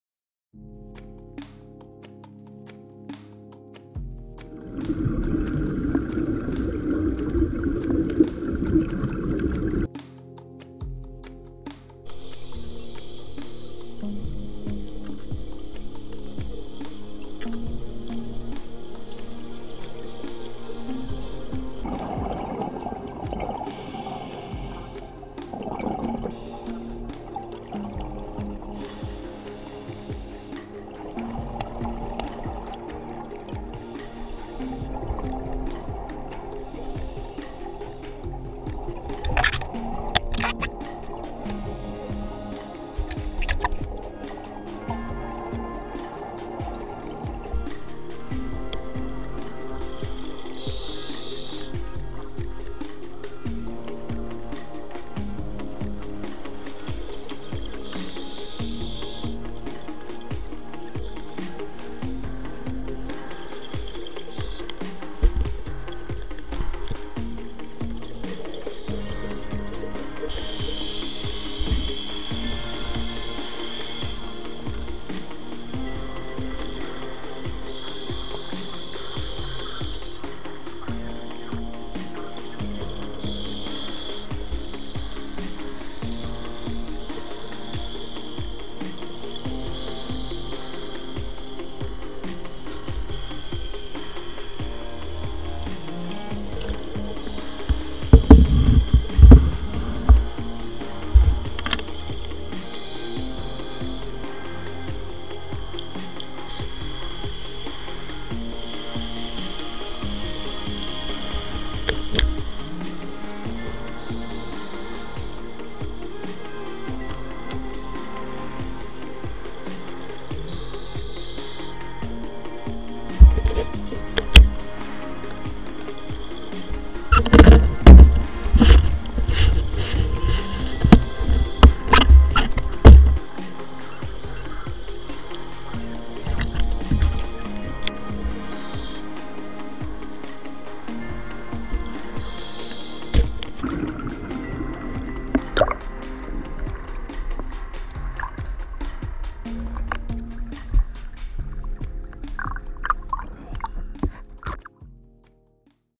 Diving the Sea Tiger Wreck - Honolulu, Hawaii - Roulette Travel
A few years ago, I took a trip to Oahu, and was lucky enough to get on a last minute trip down to the Sea Tiger wreck. A few miles offshore from Honolulu and sitting in about 120' of water (though the deck is at between 90-100'), its an adventure worthy of any diver's bucket list.

While there wasn't much coral growth at that depth, the wreck was teeming with life - including an abundance of Hawaiian Green Sea Turtles.

Most of this was shot with a GoPro Hero3.

For more adventures, reviews and guides - visit roulette-travel.com, and follow along on instagram @roulette_travel!